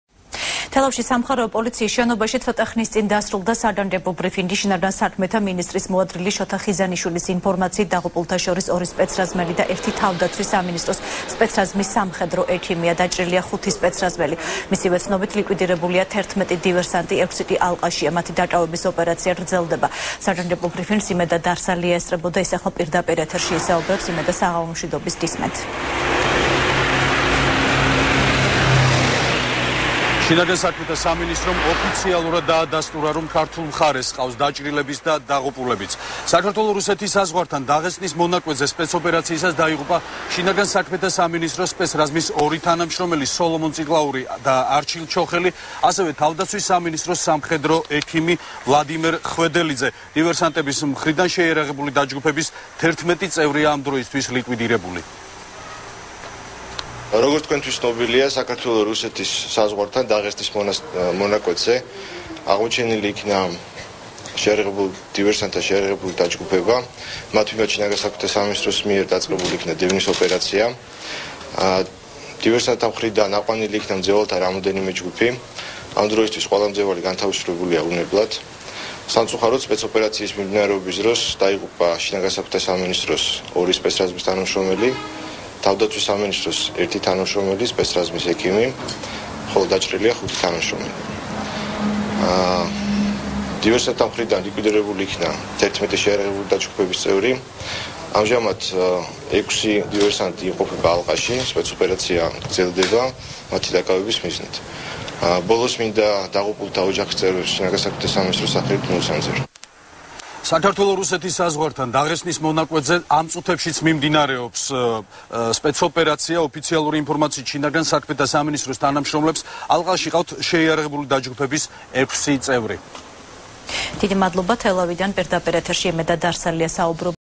ახალი 6 | შსს-ს განცხადება | 29.08.12
თელავში, სამხარეო პოლიციის შენობაში ცოტა ხნის წინ დასრულდა საგანგებო ბრიფიგი. შინაგან საქმეთა მინისტრის მოადგილის, შოთა ხიზანიშვილის ინფორმაციით, დაღუპულთა შორის ორი სპეცრაზმელი და ერთი თავდაცვის სამინისტროს სპეცრაზმის სამხედრო ექიმია. დაჭრილია ხუთი სპეცრაზმელი.  მისივე ცნობით, ლიკვიდირებულია 11 დივერსანტი, 6 კი ალყაშია. მათი დაკავების ოპერაცია გრძელდება. საგანგებო ბრიფინგს იმედა დარსალია ესწრებოდა, ის ახლა პირდაპირ ეთერში ისაუბრებს.